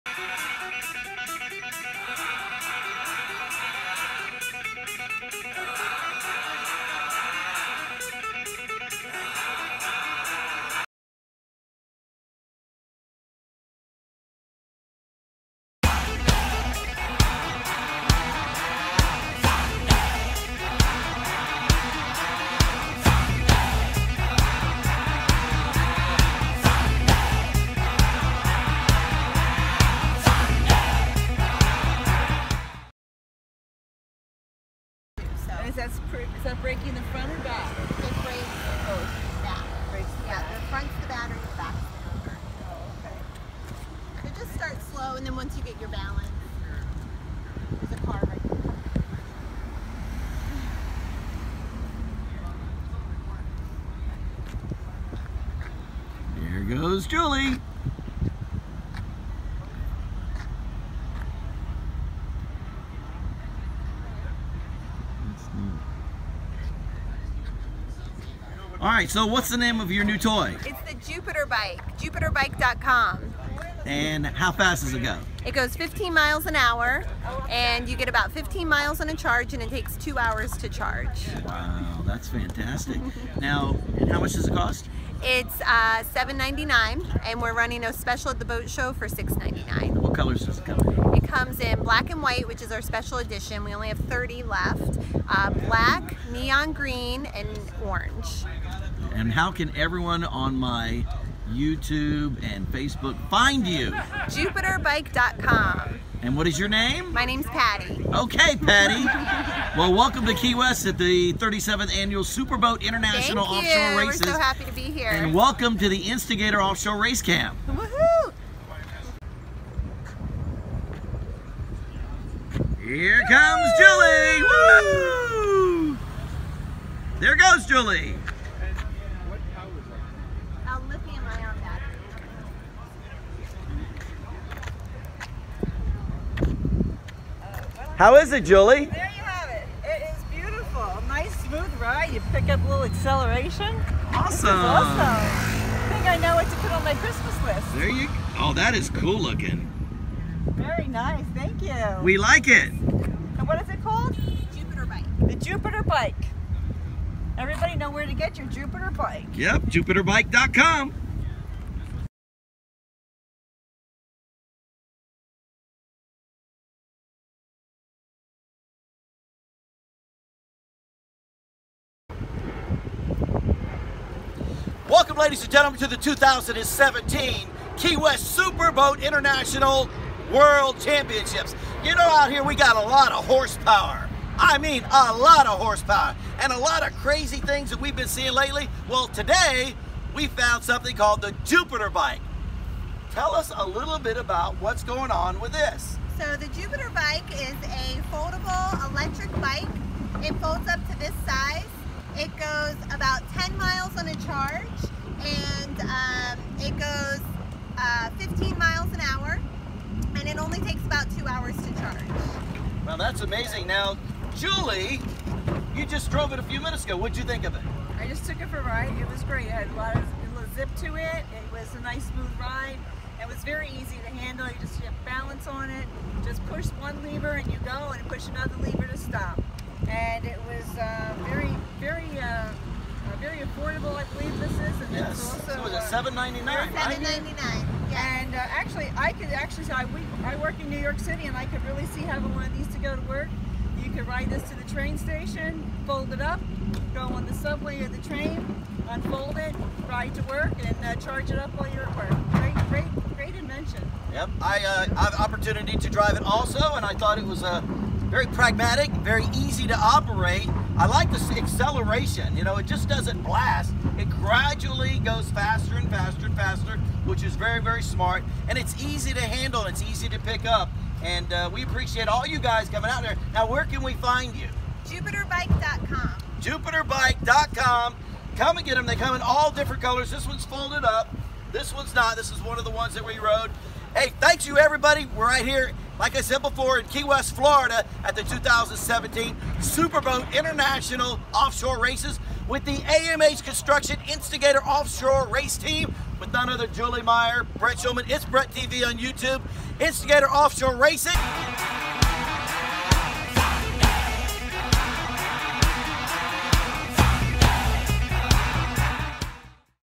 Jupiter Foldable Electric Bike arrives in Key West for the World Championships
Julie Meyer and I from the AMH Construction Instigator Offshore Racing Team get a chance to ride on the incredible new fun toy called the Jupiter Foldable Electric Bike at the 37 Annual Key West World Championship . It's fun ,easy to ride and goes anywhere !! We love it !!